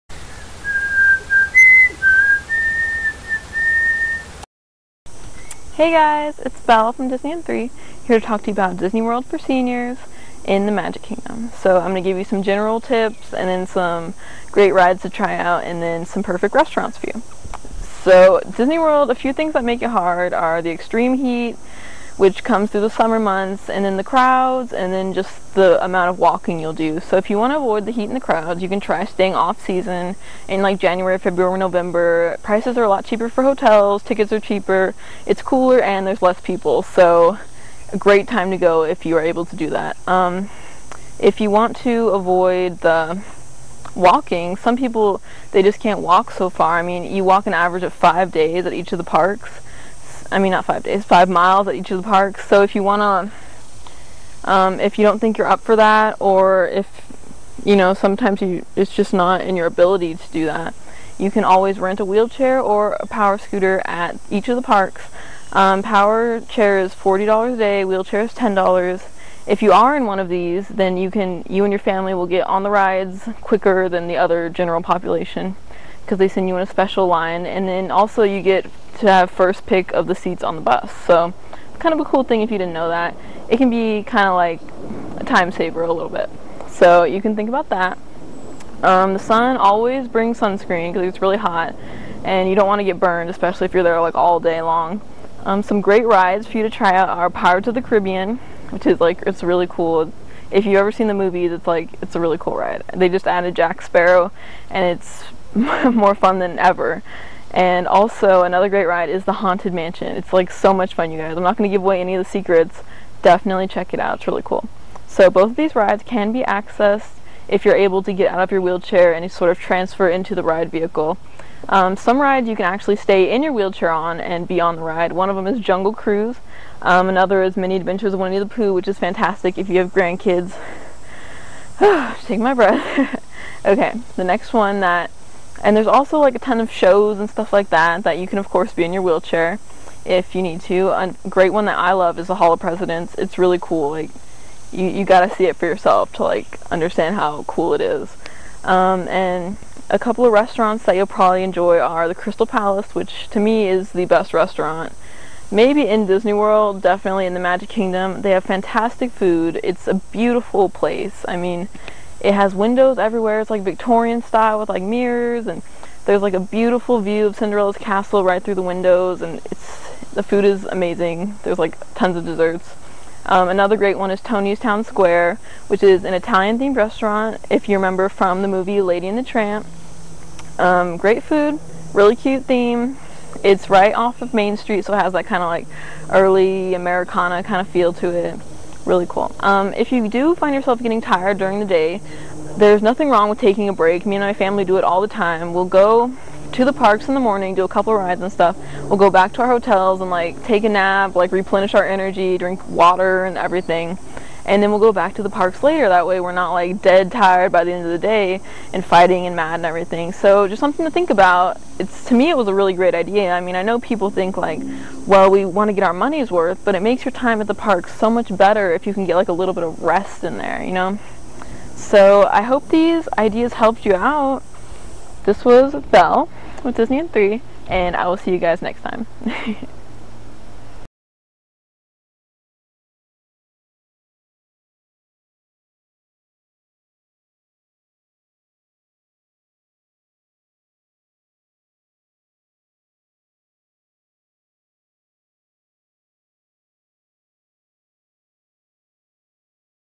Disney World for Seniors: The Magic Kingdom
Hey Guys! Just wanted to help out Seniors have fun at Disney World in The Magic Kingdom! Disney can be great fun for anyone, as much for grandparents as for little kids if you know what to do! Enjoy your trip :)